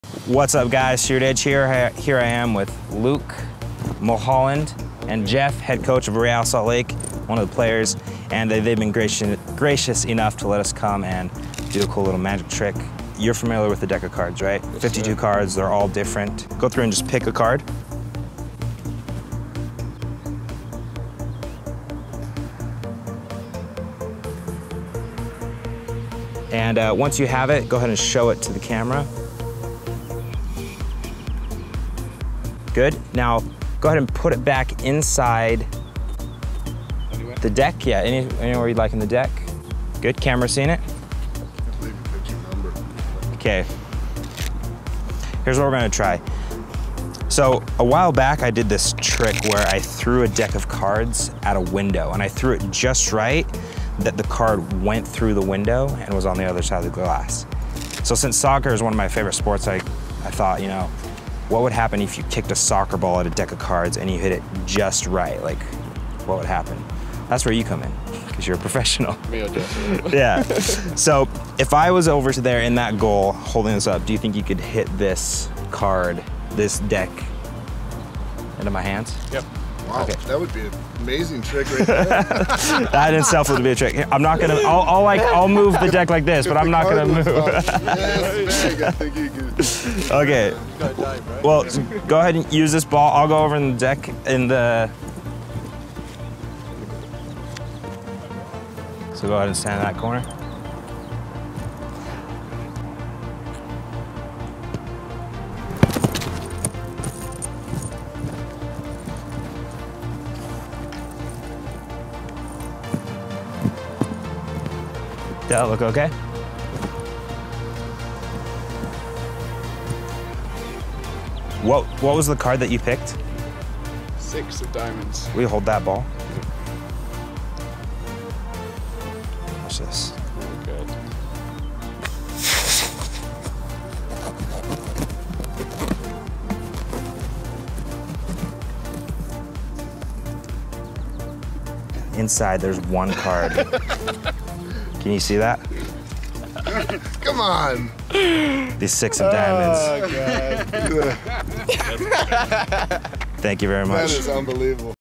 Card In Soccer Ball Magic Trick
Cutting open a soccer ball/football in this magic trick with a professional soccer player.